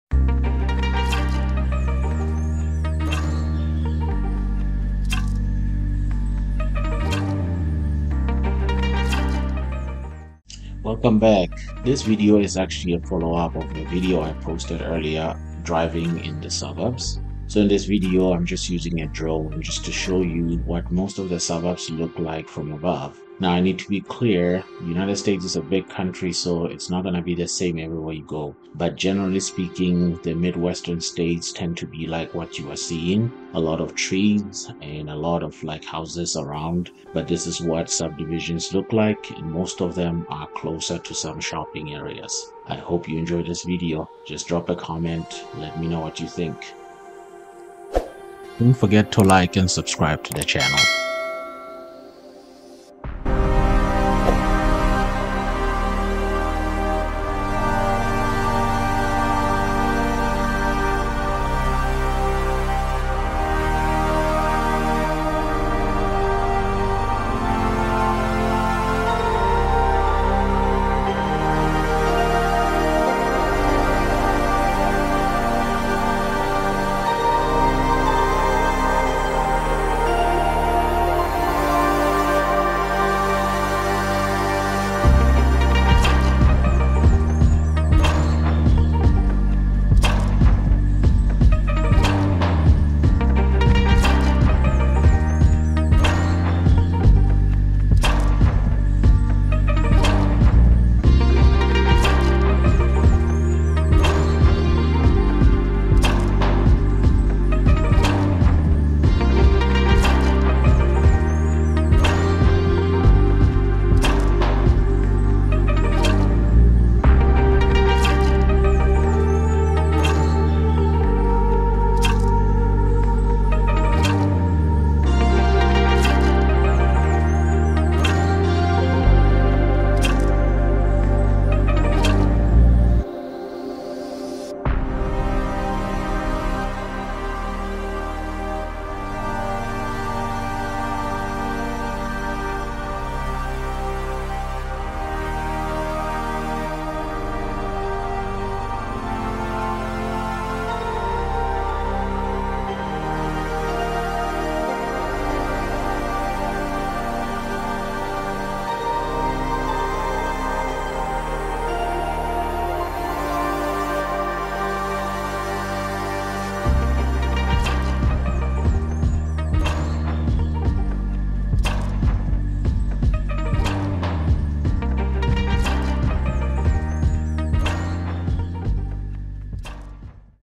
Subdivisions in the US | Part 2
The suburbs in this video are located in Greenwood and Southwestside of Indianapolis.